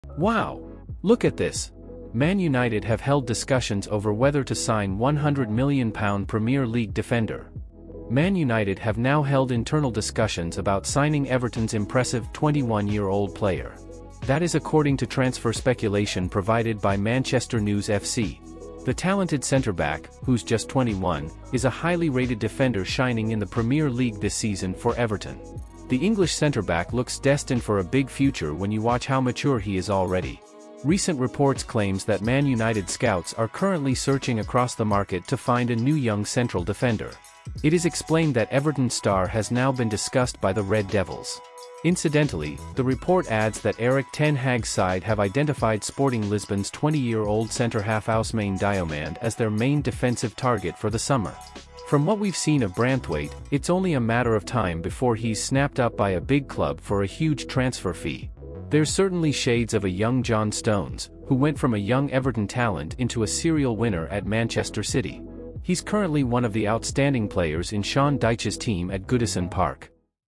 ✅ IT JUST HAPPENED! 🔴 Utd Have Held Discussions to sign 🏴󠁧󠁢󠁥󠁮󠁧󠁿 £100m EPL Defender! Man United News
✅ IT JUST HAPPENED! 🔴 Utd Have Held Discussions over Whether to sign 🏴󠁧󠁢󠁥󠁮󠁧󠁿 £100m EPL Defender! Man United News Today

🚨 1303